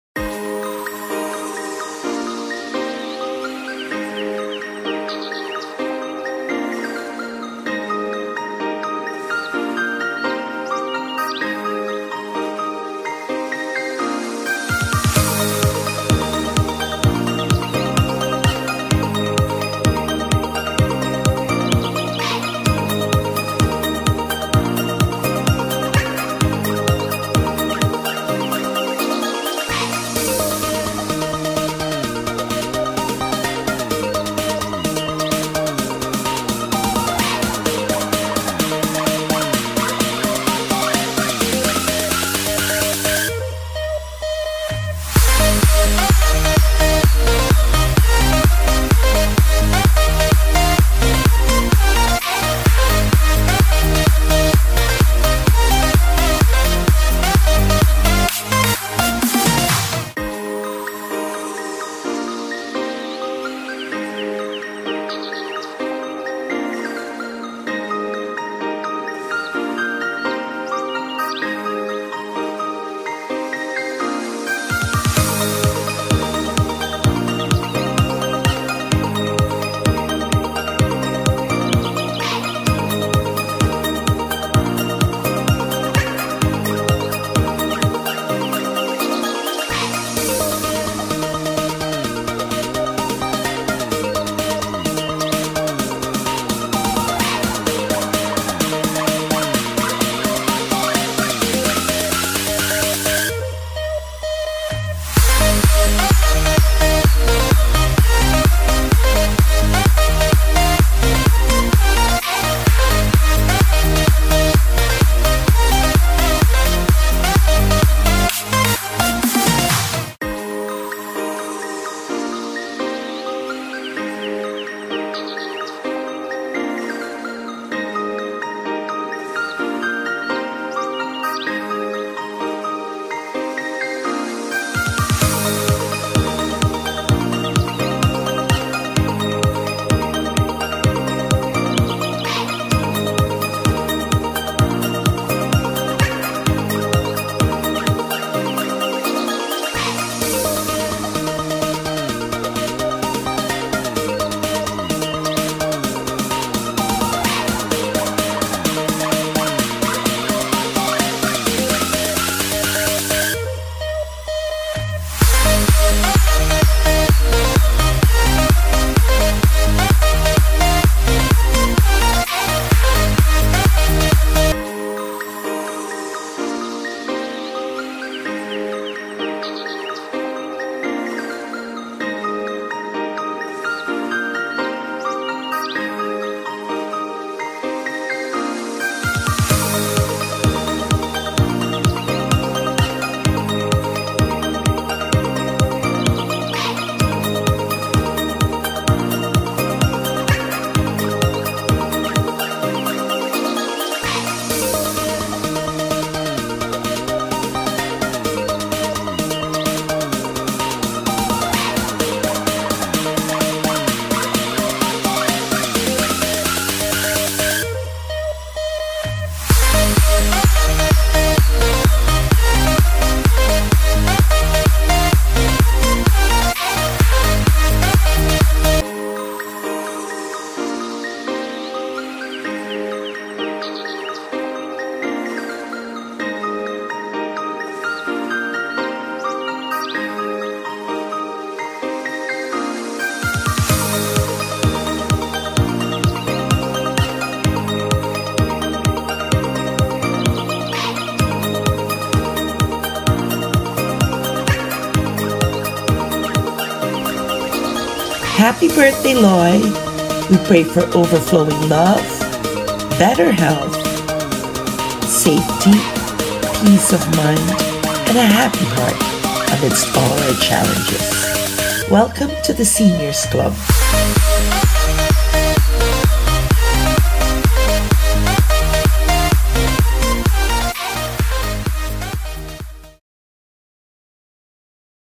ELOISA before her 60th BIRTHDAY 2023
This is an updated birthday video montage for Eloisa before she turns 60💕

Here’s a fun look back to UPCC High School, USC Bio and CDCM Medicine as well some High School reunions… and you might just be a part of this!  Enjoy💕

Happy Happy Birthday Loi —- our generous  and strong survivor. May God bless you with continued overflowing love, better health, a positive outlook and a happy heart❤️